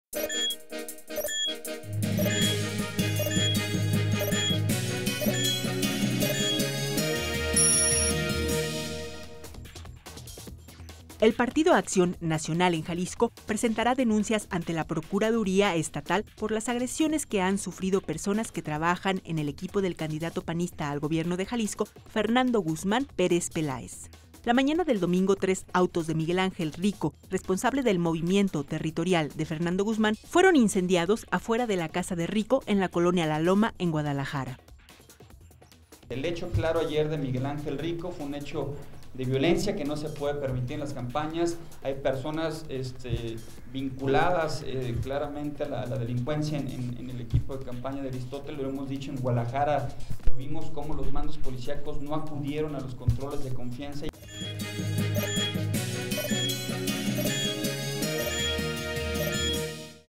Corte 44 - El PAN denunciará, por agresiones a su equipo de campaña - Mayo 21 de 2012
El Partido Acción Nacional en Jalisco presentará denuncias ante al Procuraduría  Estatal por las agresiones que han sufrido personas que trabajan en el equipo del candidato panista al Gobierno del Estado, Fernando Guzmán Pérez Peláez.

La mañana del domingo,  tres autos de Miguel Ángel Rico, responsable del movimiento territorial de Fernando Guzmán, fueron incendiados afuera de la casa de Rico, en la colonia La Loma en Guadalajara.

Informe Vanesa Robles